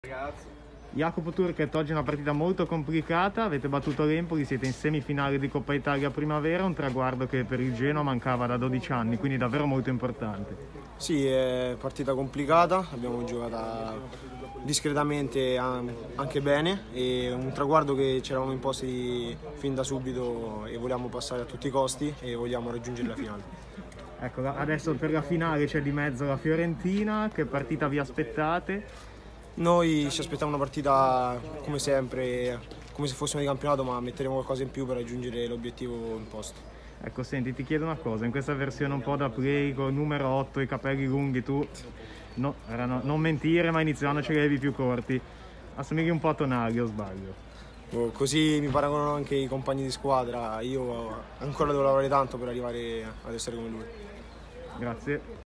Coppa Italia Primavera, Genoa in semifinale | Jacopo Turchet: “Un traguardo che ci eravamo imposti”
Partita complicata, giocata discretamente, a tratti anche bene. È un traguardo che ci eravamo imposti fin da subito di passare: volevamo raggiungere la finale". Poche parole, ma idee molto chiare al termine del 2-0 di Begato contro l'Empoli per Jacopo Turchet, oggi faro del centrocampo della Primavera rossoblu.